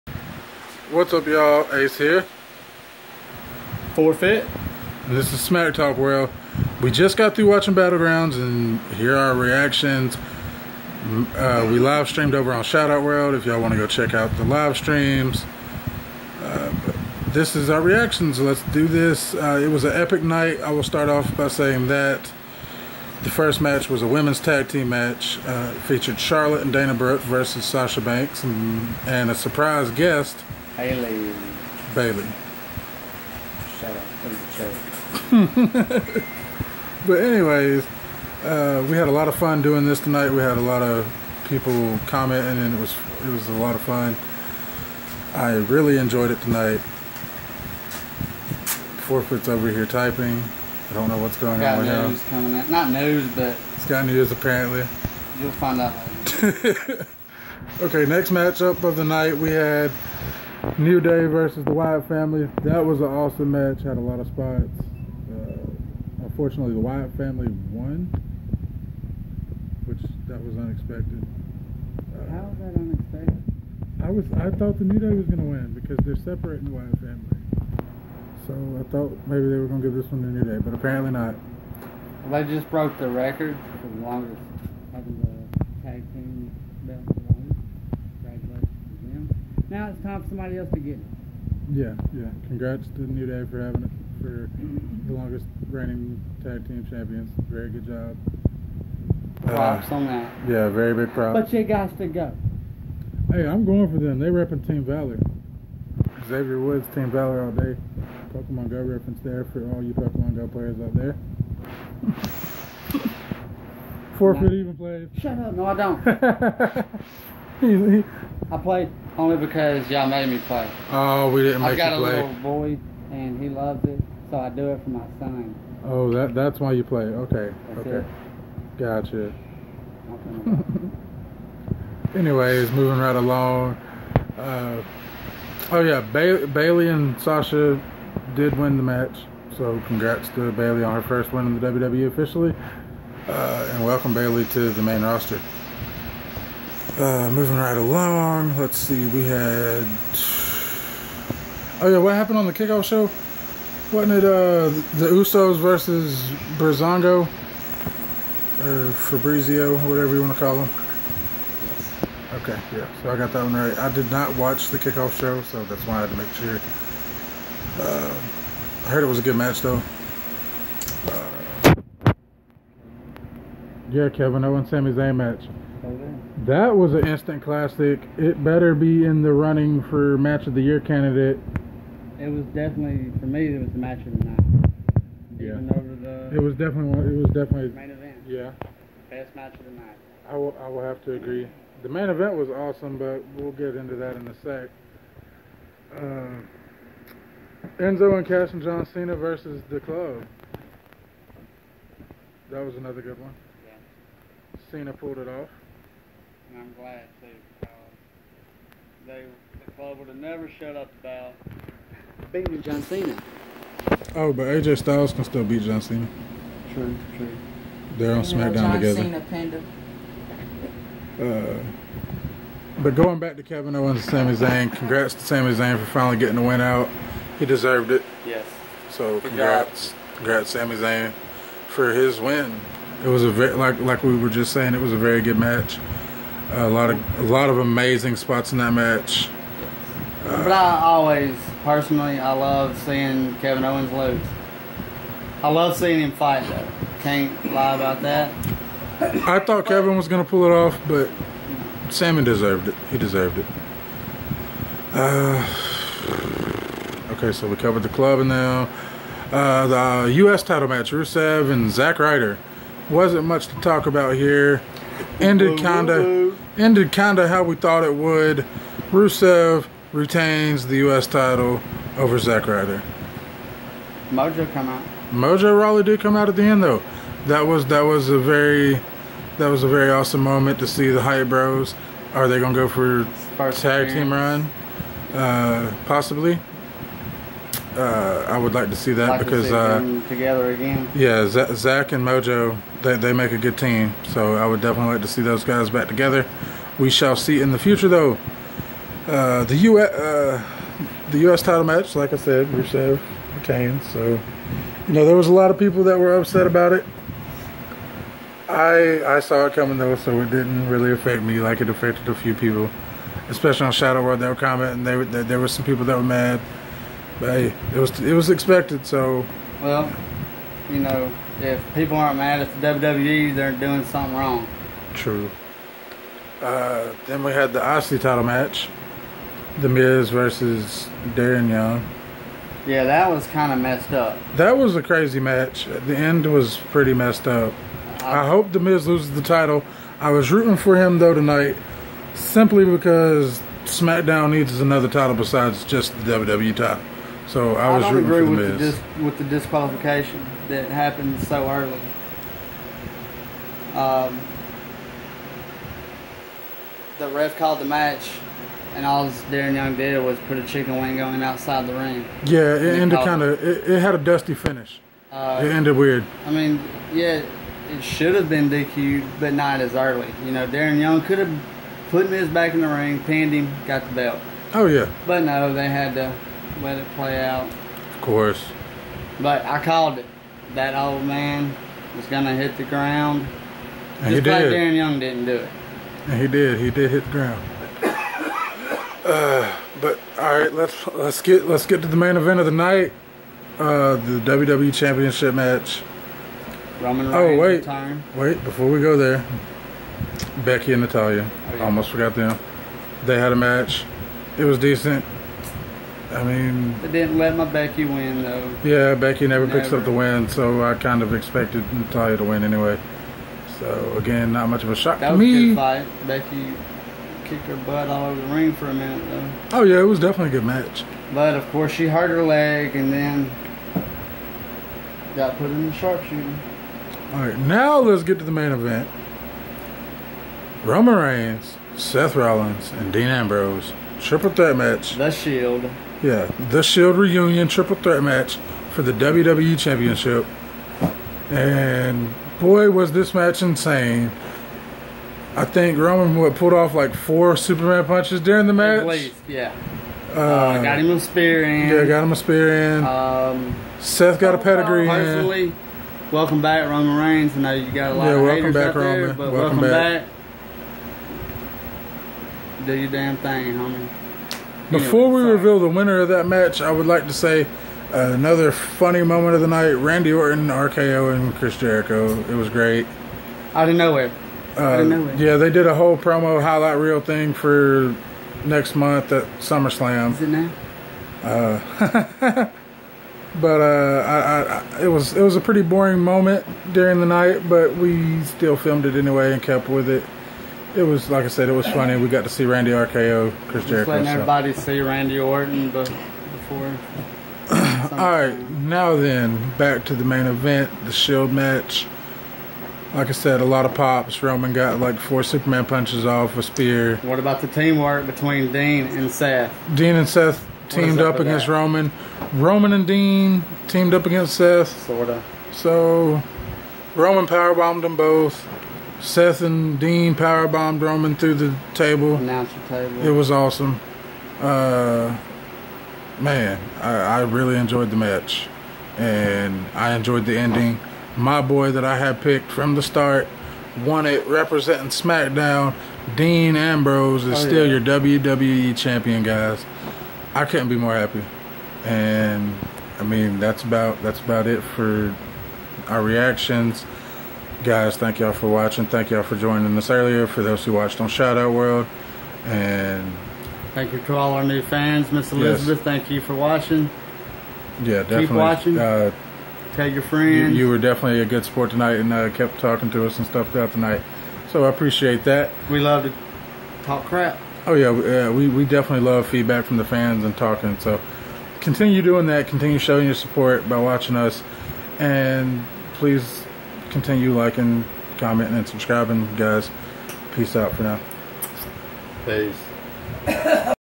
WWE Battleground 2016 reactions!
Hey guys Ace&4Phit back again! This time with our reactions to WWE Battleground! Let us know what you guys thought of the night in the comments below!